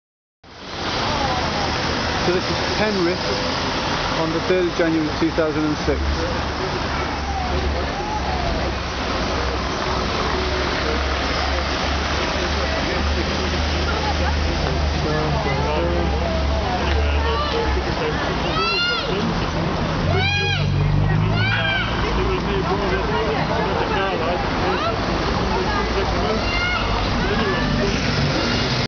Penrith
A snippet of the centre of Penrith, Cumbria, England